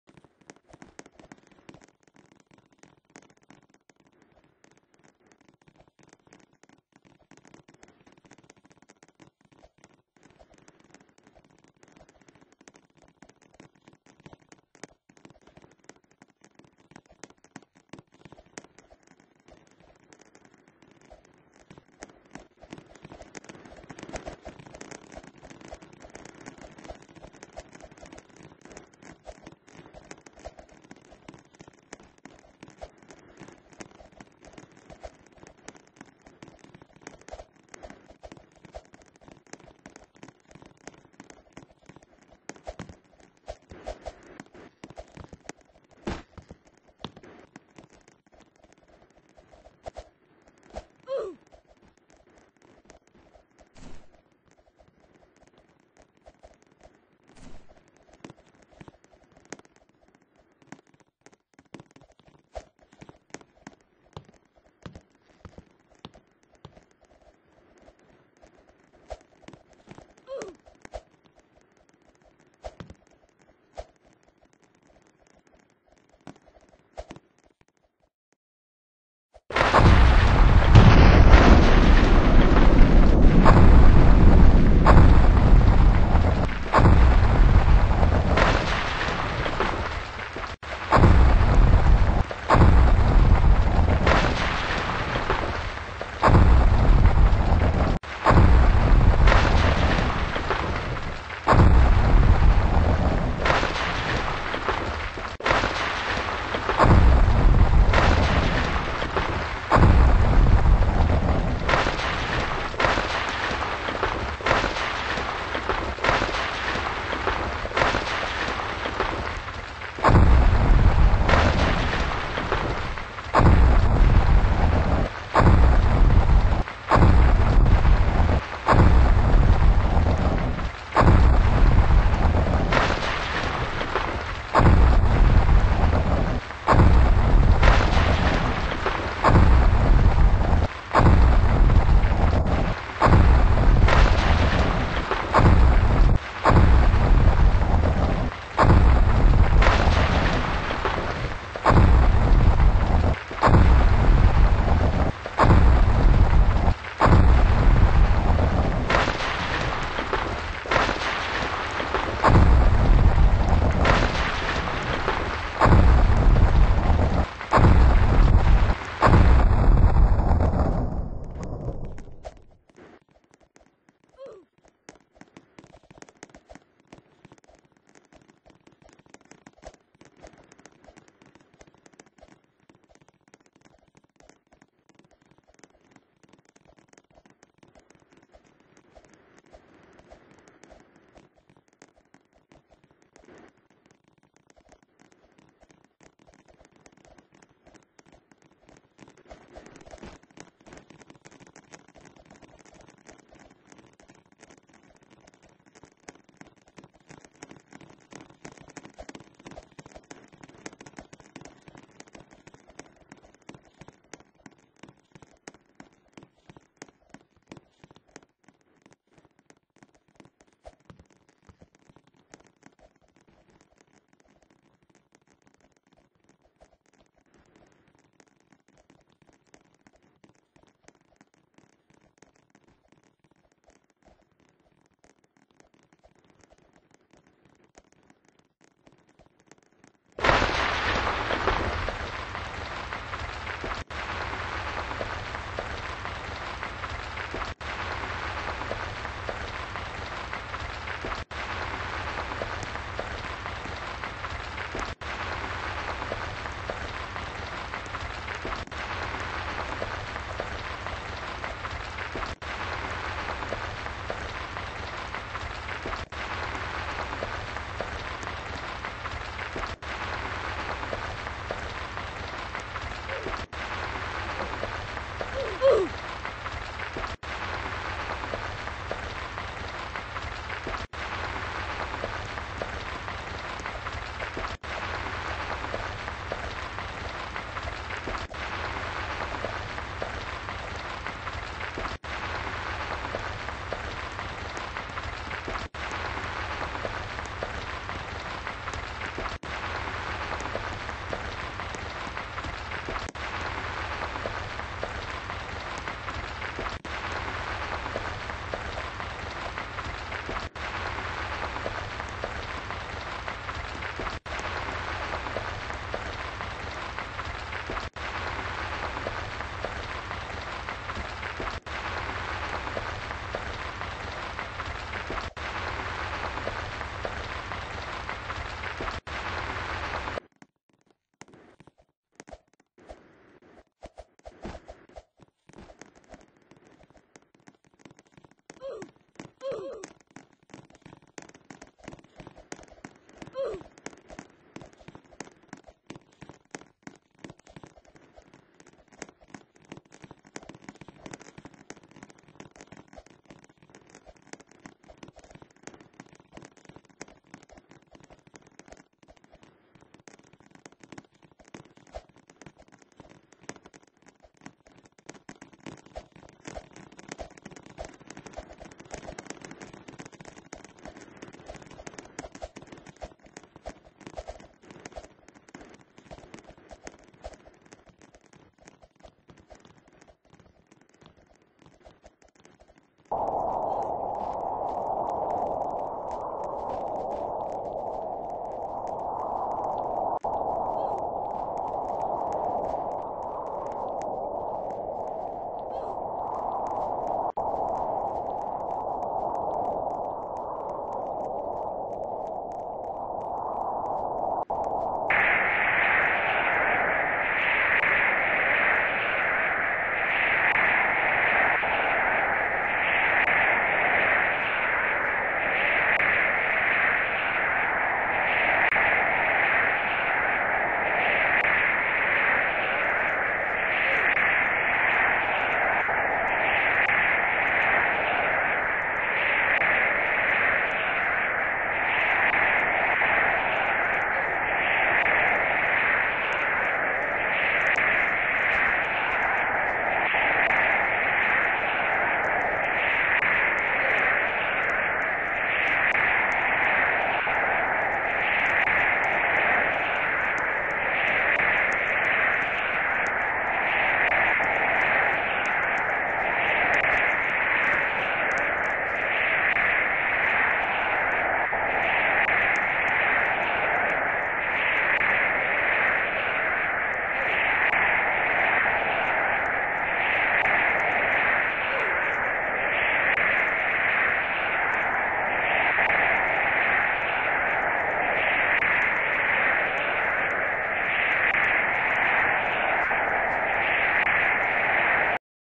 오늘은 자연재해에서 살아남기 게임!! [YouTube 카코]
오늘은 자연재해를 피해서 살아남기를 해보았어요